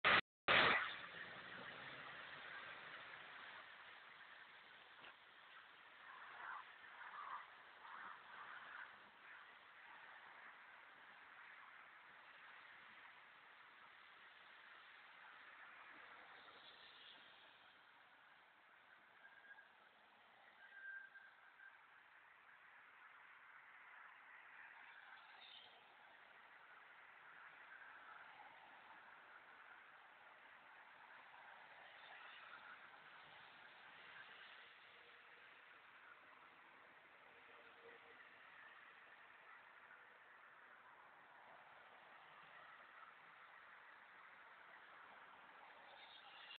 Prestigio MultiPhone PAP4300 DUO тестовое видео
"Prestigio MultiPhone PAP4300 DUO -тестовое видео на автоматических настройках и максимальном разрешении. Создано в пасмурную погоду".